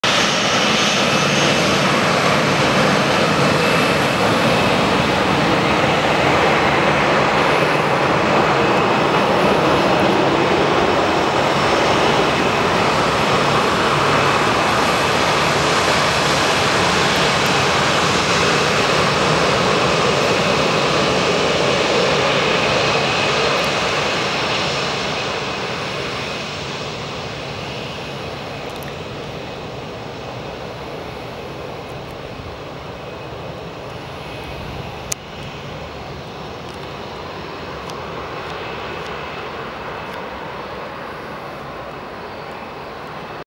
Ryanair EI-DLZ - part 2 (MOVIE - ONLY TO TEST)
EI-DLZ - landing - 17.08
FR4531 - Sandefjord/Torp - Bergamo/Milan